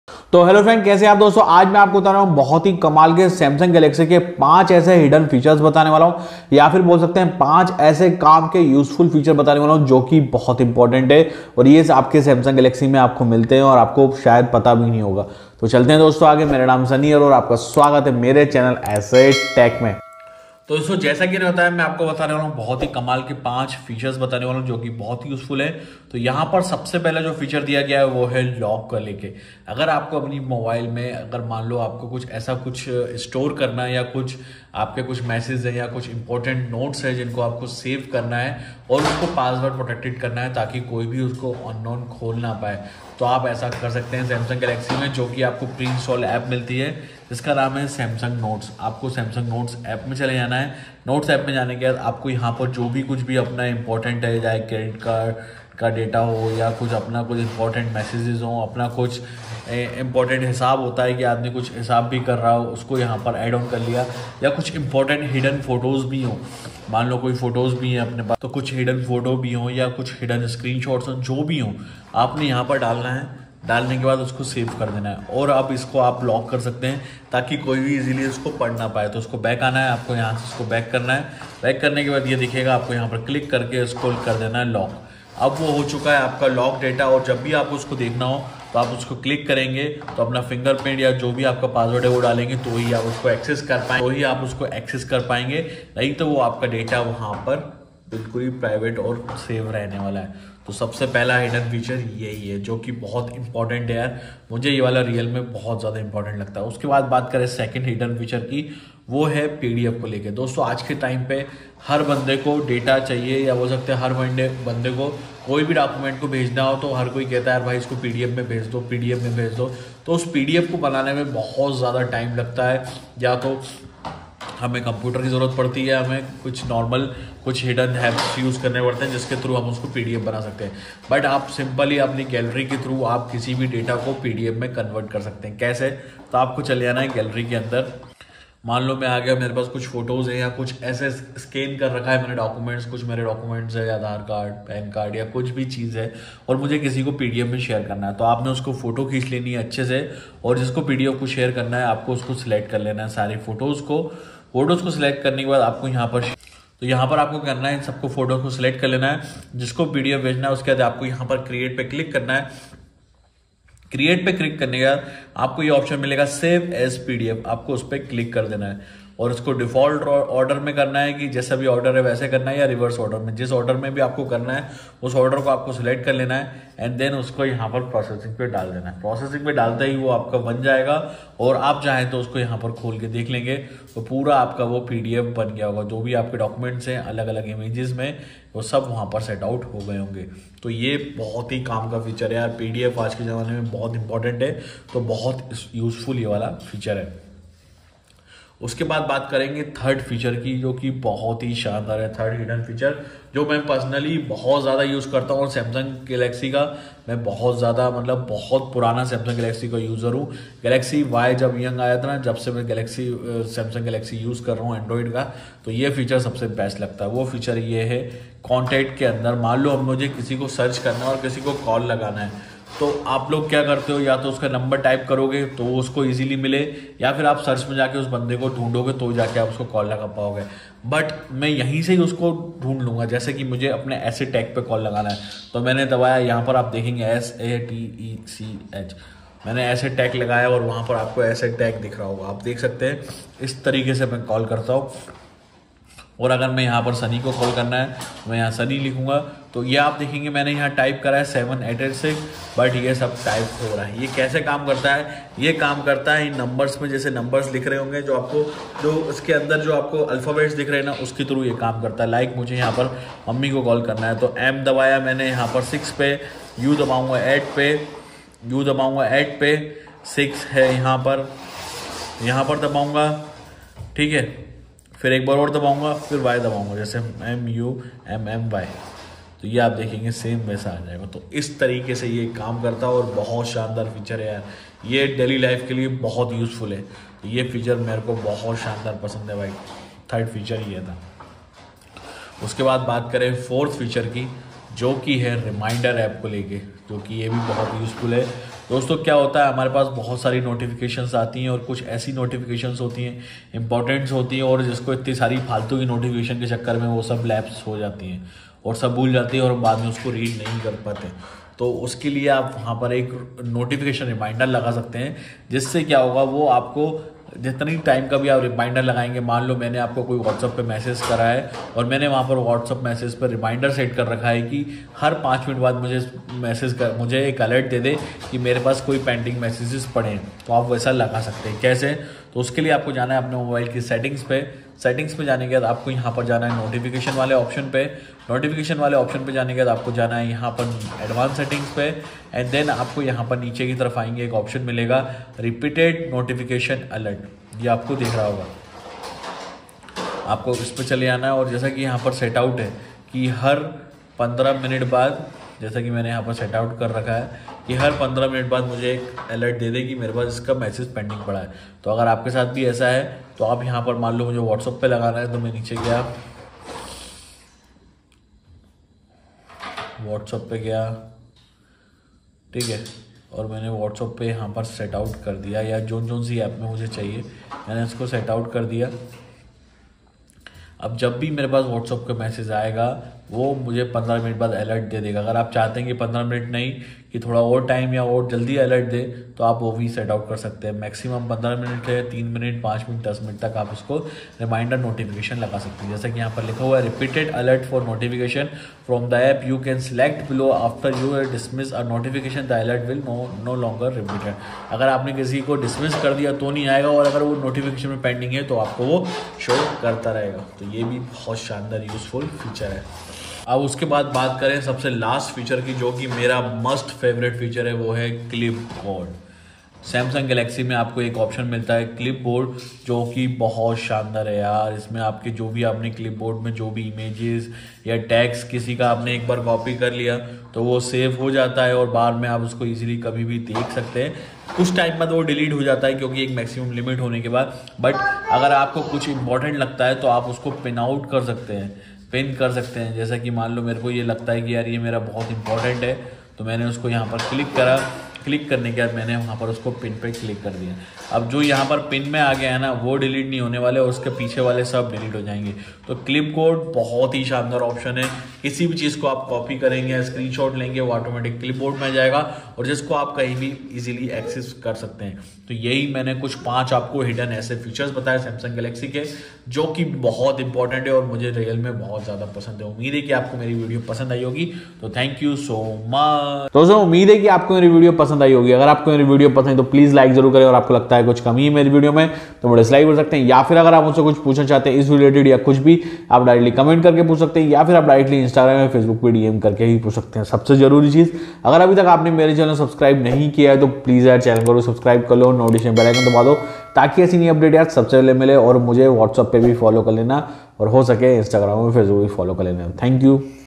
5 Must Know Hidden Features for Samsung Galaxy
in this video i describe about 5 samsung galaxy hidden features